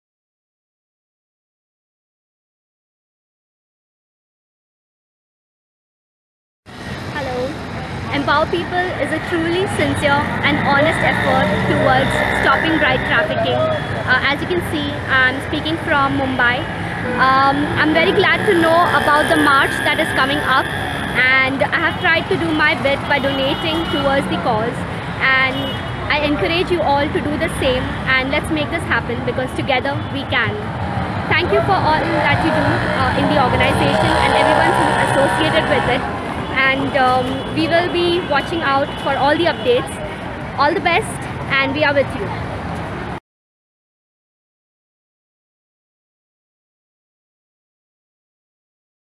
Reema message for MABT2018
With aim to develop a mass movement against Modern Day slavery and engage different stake holder a caravan of activists will start a March from Karbialong District of Assam and would culminate in Chandigarh. The marchers will organize public meetings in bazaar-haats and seminars with School and college going students in the institution premises. The March will cover at least 8000 odd kilometers in 6 states, more than 1000 high schools more 100 colleges and other institutions and least but not last 2000 bazaar and hats.